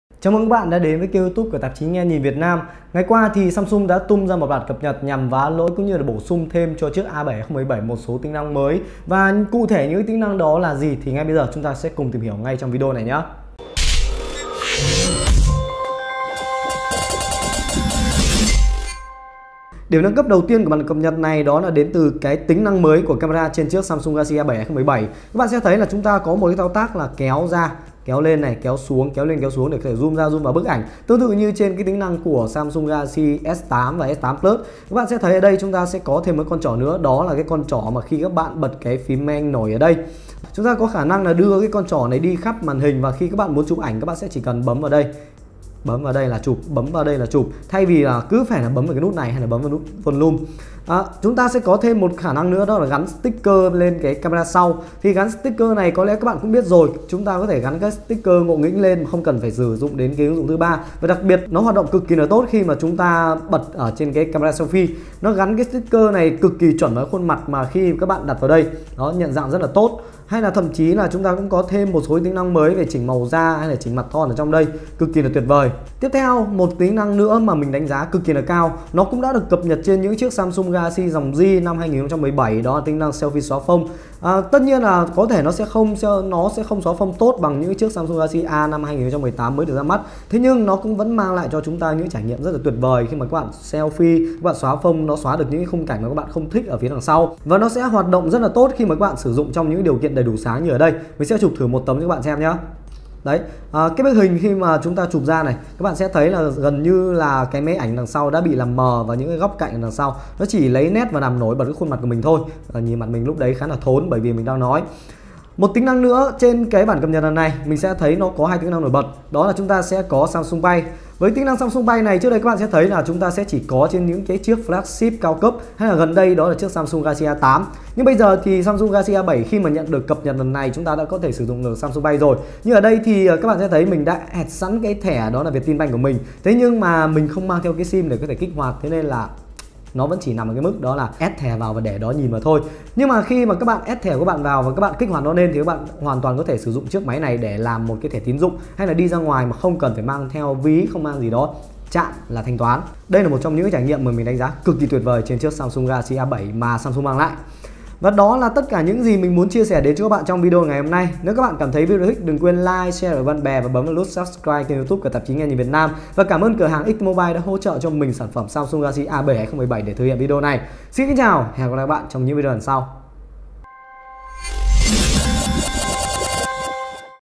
A7 2017 có được tính năng đặc biệt gì khi nhận update phiên bản mới? - Nghenhinvietnam.vn
A7 2017 nhận được cập nhật mới với các tính năng cực kỳ hữu ích, vậy tính năng đó là gì thì ngay sau đây các bạn hãy cùng nghenhinvietnam tìm hiểu trong video này nhé !!!

Cảm ơn XTmobile đã hỗ trợ sản phẩm để nghenhinvietnam thực hiện video